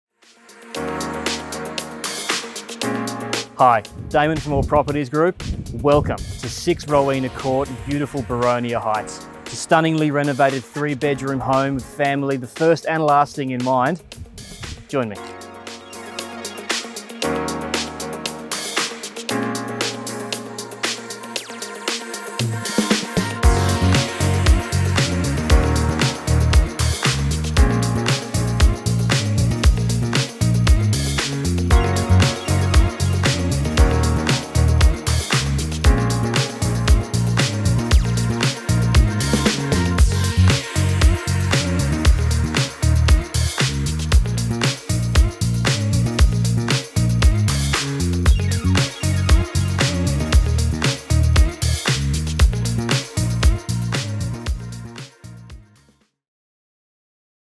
6 Rowena Court, Boronia Heights | House Tour | Daemen Bray
Daemen Bray Presents this 3 bedroom, 2 bathroom home in Boronia Heights!